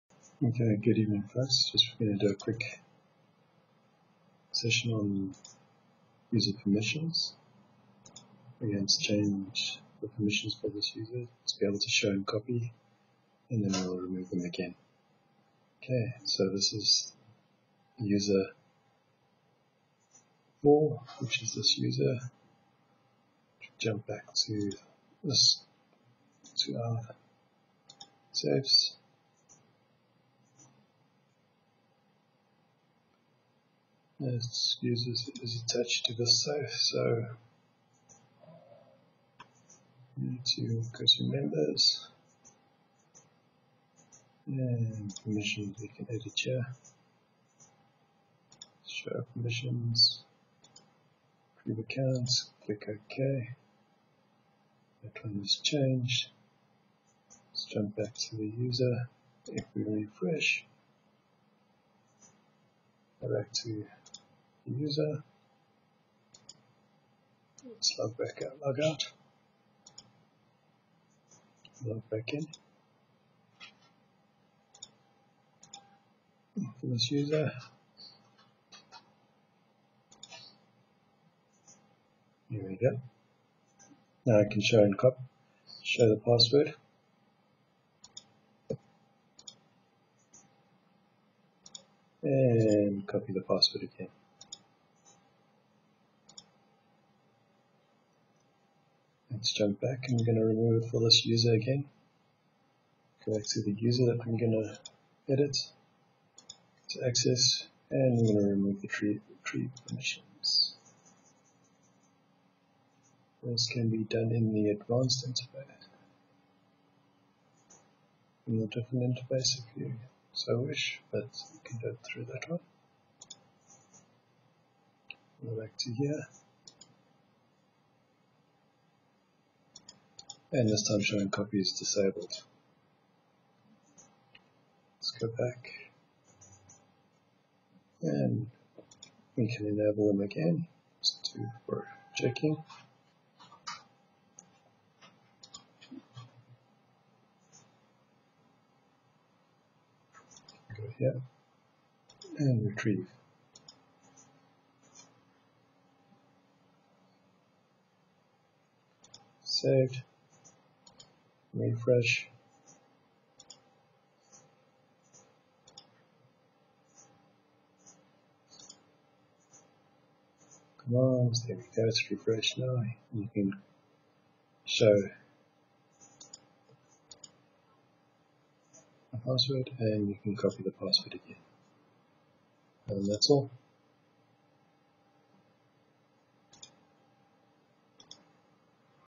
CyberArk remove show and copy for a user
Just a quick video on how to add and remove show and copy for a user.
In this example going to do this for the user with the privileged account user-adm.
Not all my servers are needed for this demo so they will not be running.